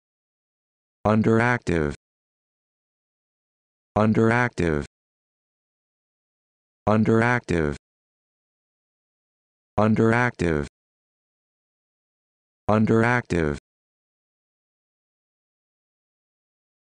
How to Pronounce Underactive
This video shows you how to pronounce Underactive